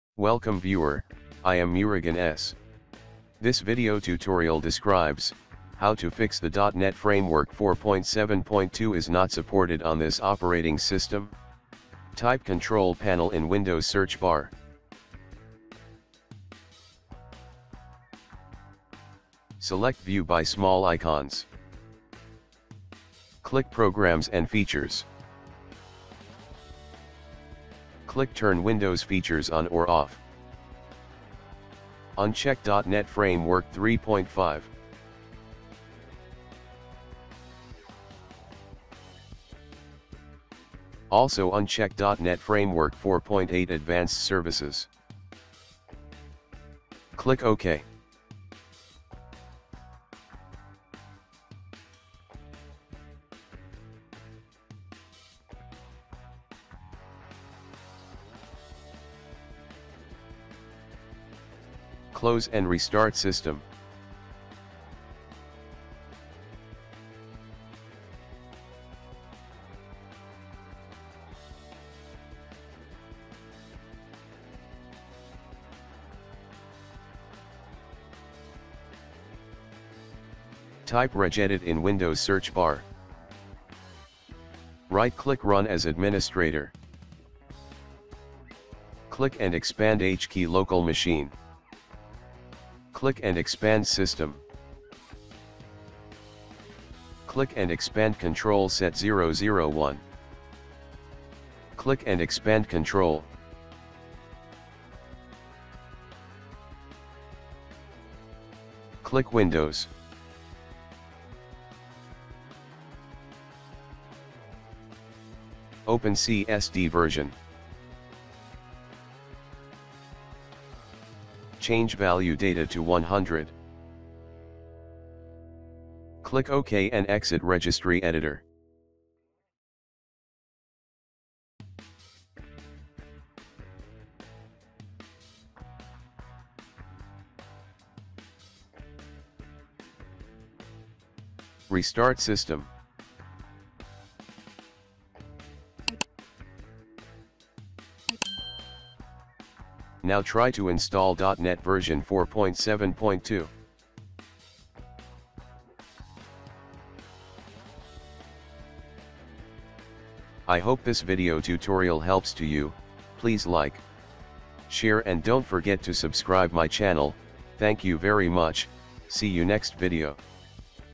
How To Fix The .NET Framework 4.7.2 Is Not Supported On This Operating System
This Tutorial Helps to How To Fix The .NET Framework 4.7.2 Is Not Supported On This Operating System

#.NetFramework4.7.2InstallError